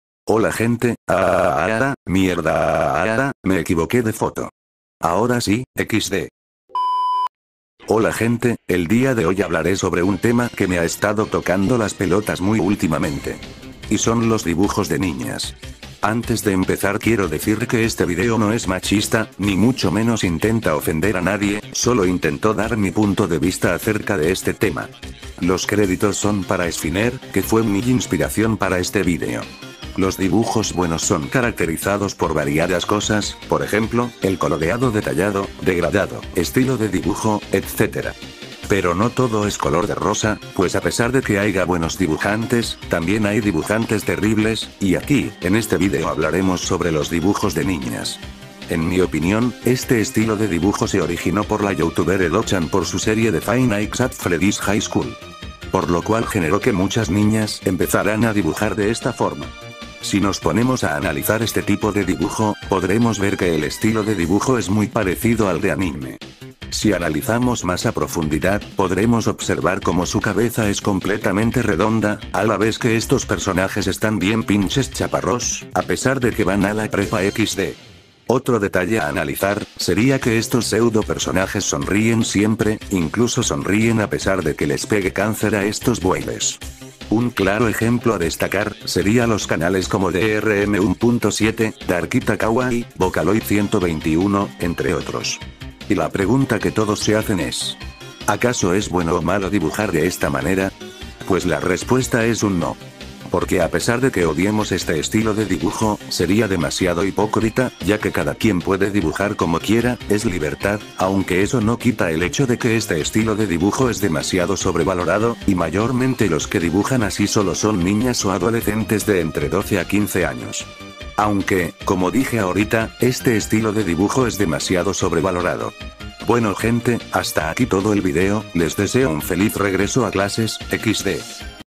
(Fail) Dibujos De Niñas Opinión (De Rad Luza)
Este Video Lo Pongo En Fail, Porque En Todo El Vídeo Estuvo Resumiendo Y Dando Datos Innecesarios Que No Aportan Nada En Su Opinión, De Echo, En Todo El Vídeo, En Tan Solo 30 Segundos O Menos Dijo Su Opinión, Es Un Video Que No Aporta Nada A Los Dibujantes, Y Espero Que El Usuario, Rad Luza, No Se Vaya A Molestar Por Poner Su Video En Fail, Pero En Serio Es Un Mal Video